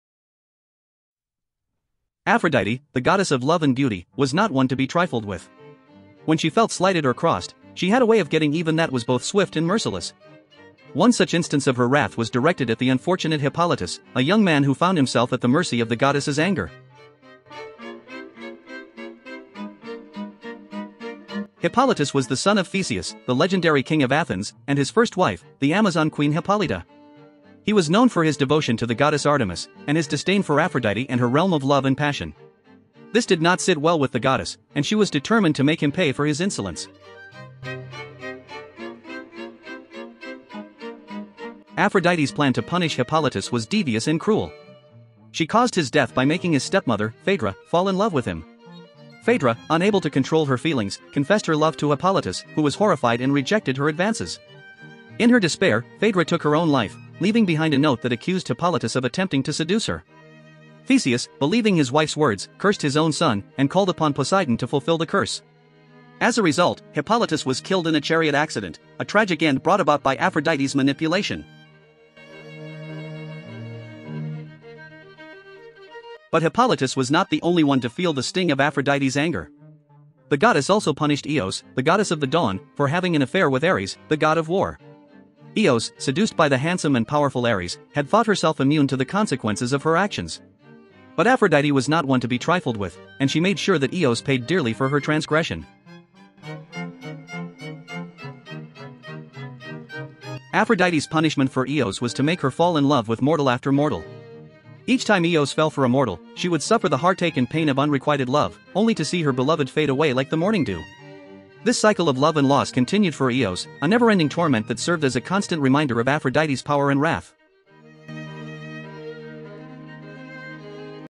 Who did Aphrodite punish? Greek Mythology Story
Aphrodite, the goddess of love and beauty, was not one to be trifled with. When she felt slighted or crossed, she had a way of getting even that was both swift and merciless. One such instance of her wrath was directed at the unfortunate Hippolytus, a young man who found himself at the mercy of the goddess's anger.

Hippolytus was the son of Theseus, the legendary king of Athens, and his first wife, the Amazon queen Hippolyta. He was known for his devotion to the goddess Artemis, and his disdain for Aphrodite and her realm of love and passion. This did not sit well with the goddess, and she was determined to make him pay for his insolence.

Aphrodite's plan to punish Hippolytus was devious and cruel. She caused his death by making his step-mother, Phaedra, fall in love with him. Phaedra, unable to control her feelings, confessed her love to Hippolytus, who was horrified and rejected her advances. In her despair, Phaedra took her own life, leaving behind a note that accused Hippolytus of attempting to seduce her. Theseus, believing his wife's words, cursed his own son, and called upon Poseidon to fulfill the curse. As a result, Hippolytus was killed in a chariot accident, a tragic end brought about by Aphrodite's manipulation.

But Hippolytus was not the only one to feel the sting of Aphrodite's anger. The goddess also punished Eos, the goddess of the dawn, for having an affair with Ares, the god of war. Eos, seduced by the handsome and powerful Ares, had thought herself immune to the consequences of her actions. But Aphrodite was not one to be trifled with, and she made sure that Eos paid dearly for her transgression.

Aphrodite's punishment for Eos was to make her fall in love with mortal after mortal. Each time Eos fell for a mortal, she would suffer the heartache and pain of unrequited love, only to see her beloved fade away like the morning dew. This cycle of love and loss continued for Eos, a never-ending torment that served as a constant reminder of Aphrodite's power and wrath.

The stories of Hippolytus and Eos serve as cautionary tales of the dangers of crossing the goddess of love. Aphrodite was not to be underestimated, and those who dared to defy her or her domain of love and passion would face dire consequences. Whether it was through manipulation, tragedy, or heartache, Aphrodite made sure that her vengeance was swift and severe.

In the end, the fates of Hippolytus and Eos serve as reminders of the power and capriciousness of the gods, and the folly of defying their will. It is a lesson that has echoed through the ages, a warning to all who would dare to challenge the authority of the divine. For when it comes to the gods, their anger is not to be taken lightly, and the consequences of their wrath can be both tragic and everlasting.